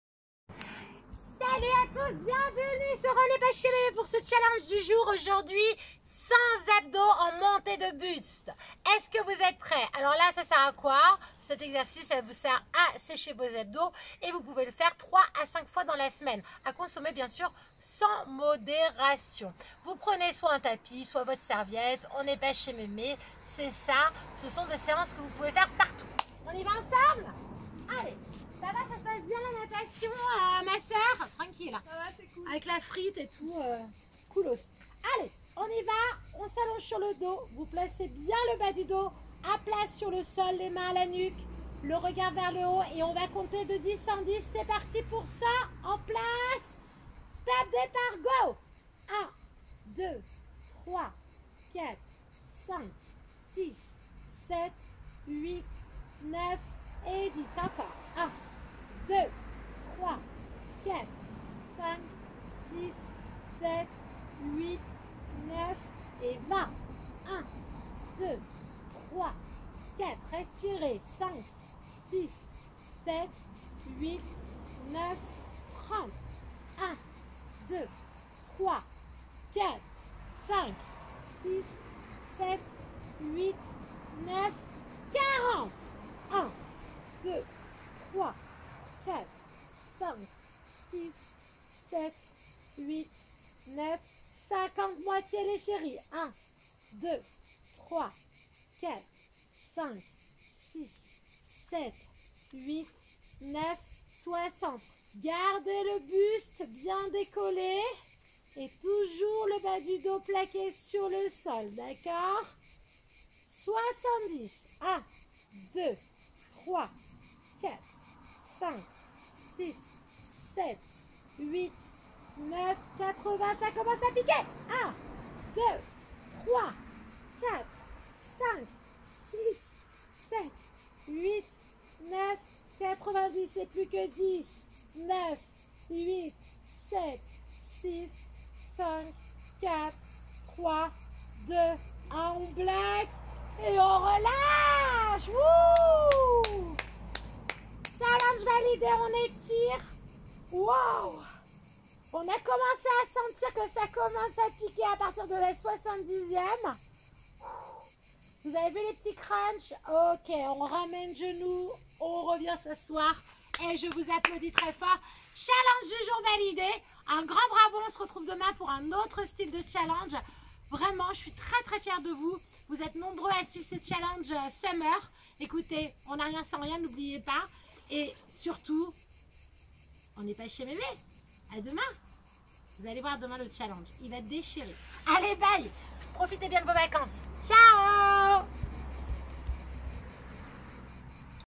challenge ventre plat 100 abdos crunch by ONPCM
Venez partager ce challenge spécial Abdos ventre plat avec la méthode On N'est Pas Chez Mémé.

Des routines rapides et efficaces.